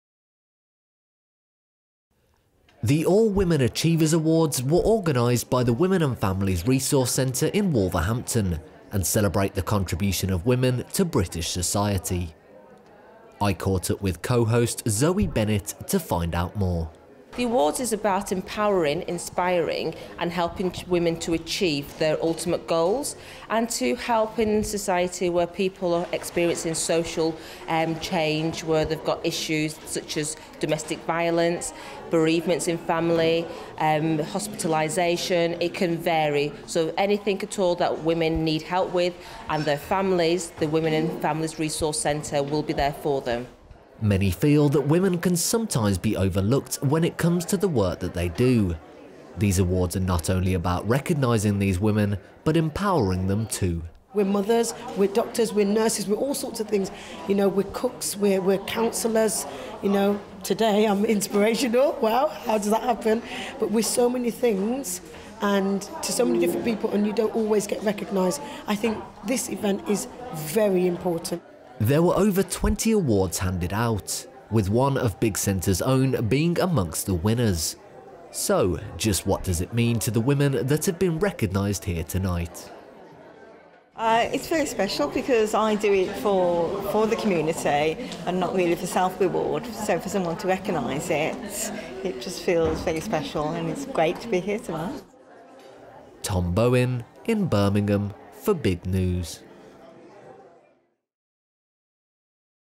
Big News - The All Women Achievers Awards 2016
4th July 2016. Organised by The Women and Families Resource Centre, The All Women Achievers Awards, highlights and creates awareness of ordinary women from different platforms  who are doing extraordinary things within their communities in the United Kingdom.  Tom Bowen reports.